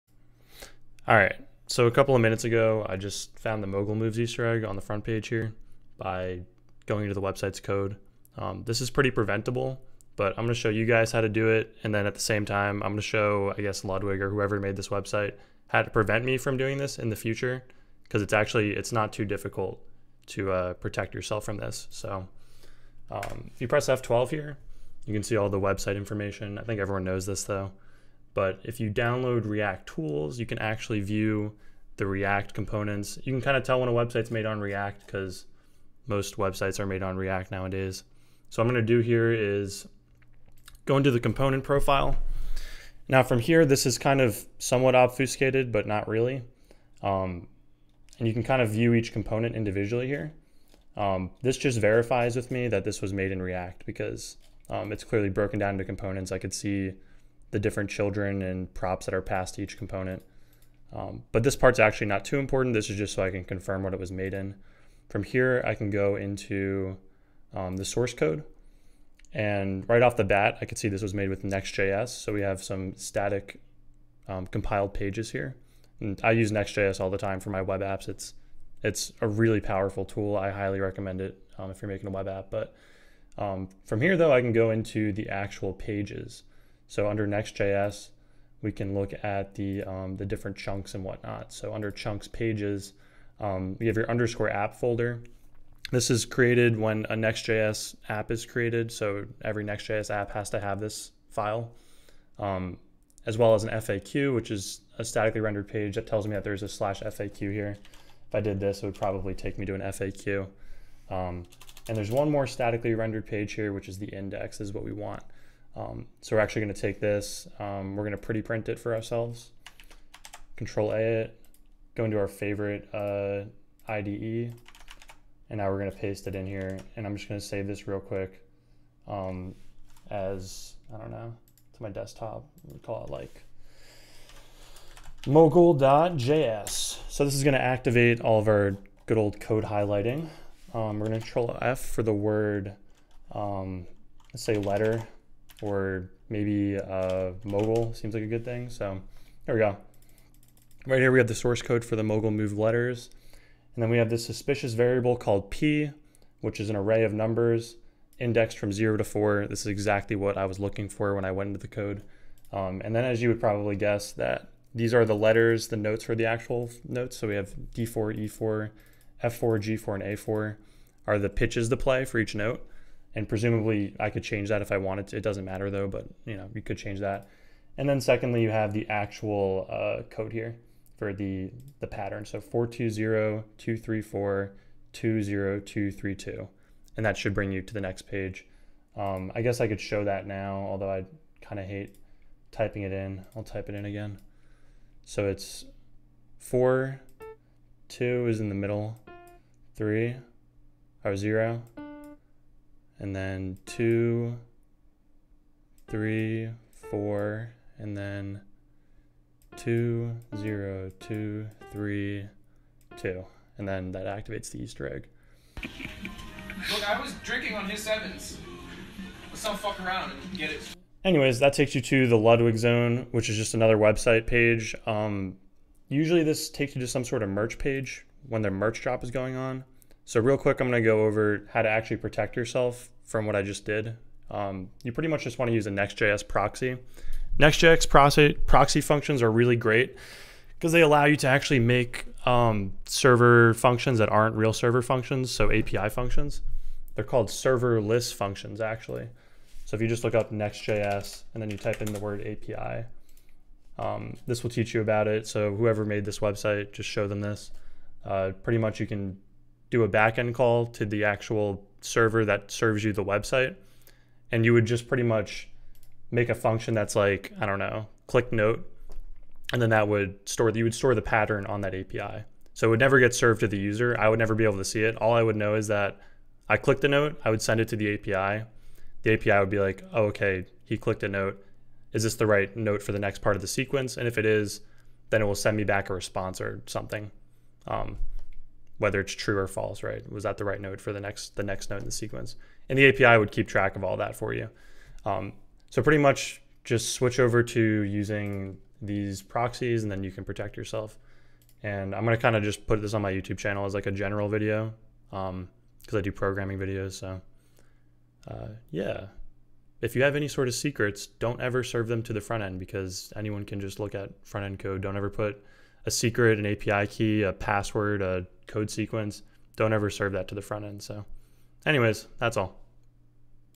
How to find an easter egg on a website and how to prevent it.
Found the ludwig.gg easter egg, thought it'd be cool to make a quick video on it so others can try it out and prevent it.